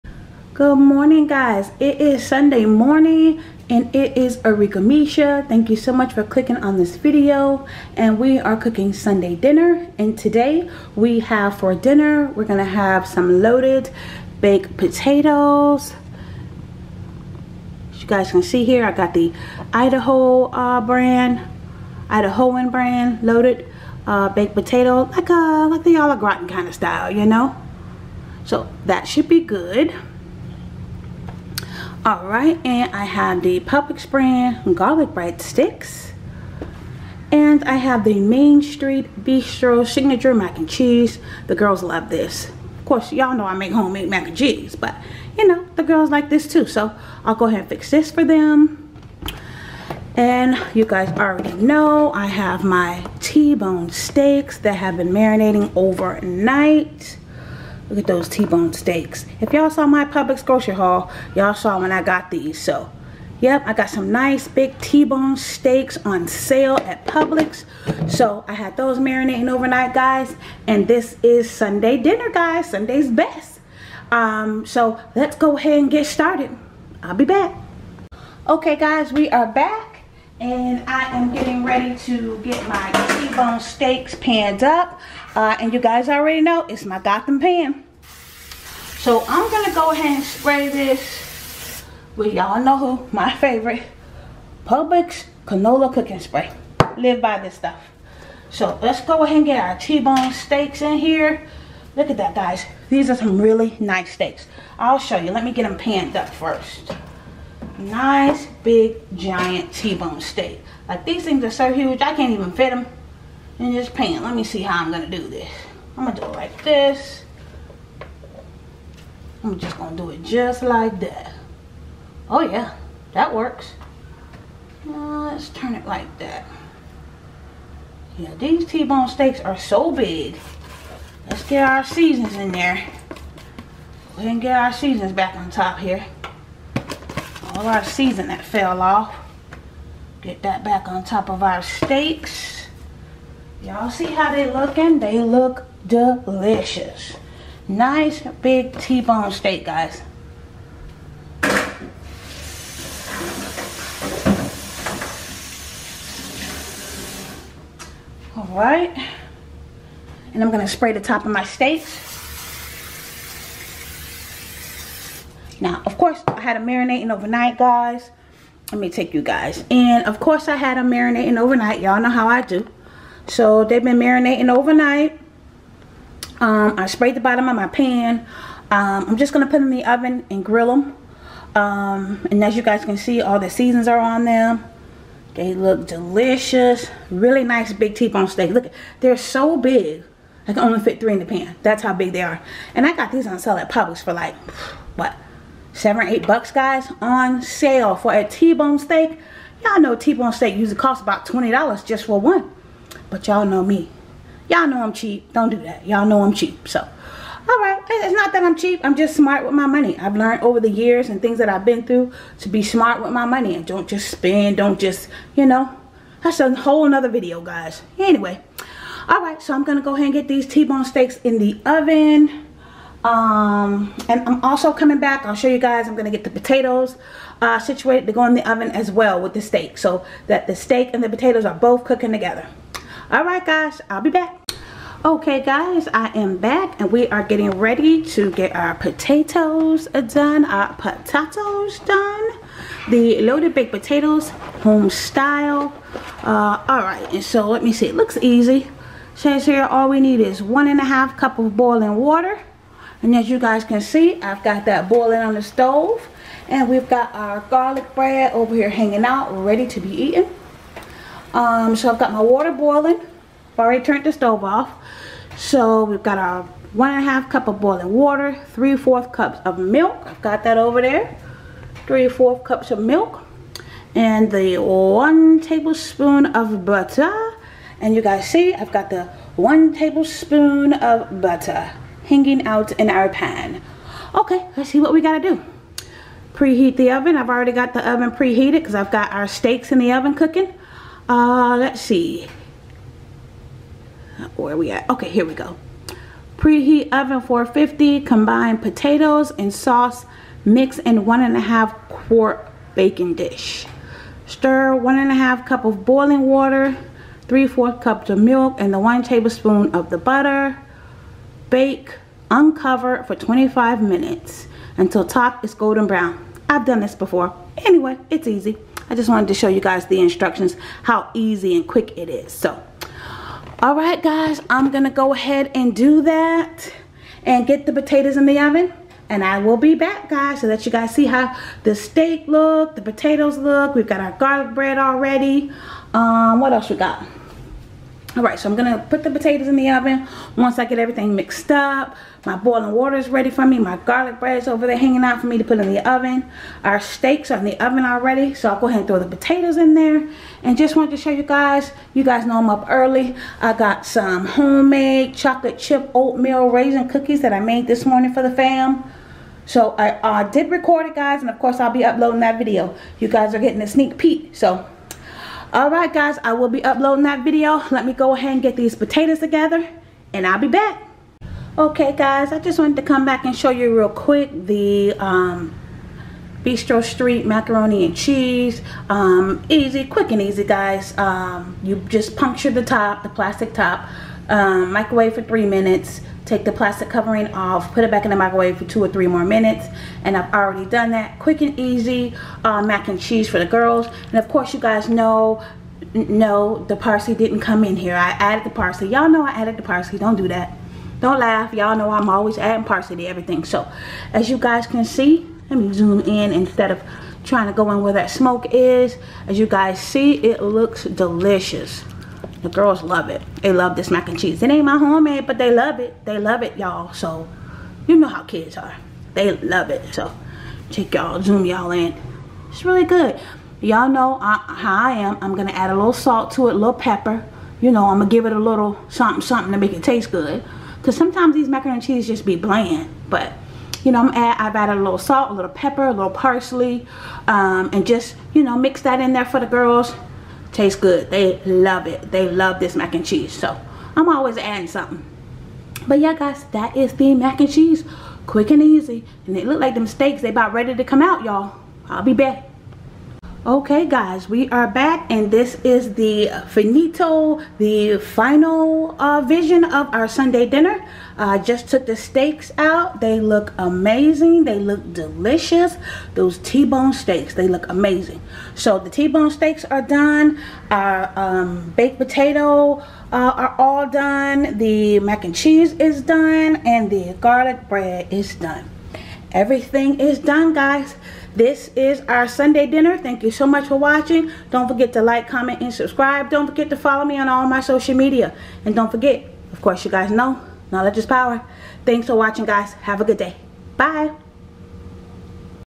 T- Bone Steaks | Loaded Potatoes | Arickamisha
Sunday dinners are always the best, I love cooking for the family. We love eating dinner and spending time together around the table talking and laughing.